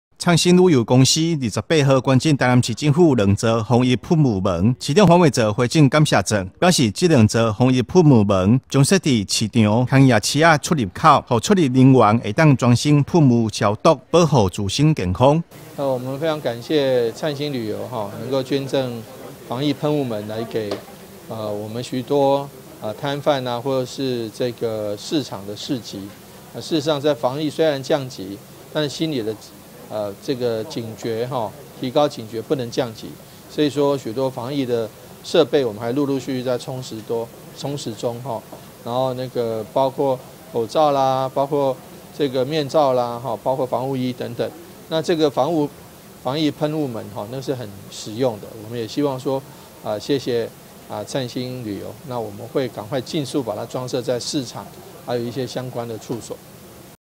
燦星旅遊捐贈南市防疫噴霧門 警戒降級市場夜市防疫更升級
企業與政府攜手守護台南市民健康與安全，28日燦星旅遊公司捐贈台南市政府兩座防疫噴霧門，台南市長黃偉哲親自回贈感謝狀，表示這兩座防疫噴霧門預計設置於市場及夜市出入口處，讓進出人員直接進行全身噴灑消毒，快速方便，加強防疫強度。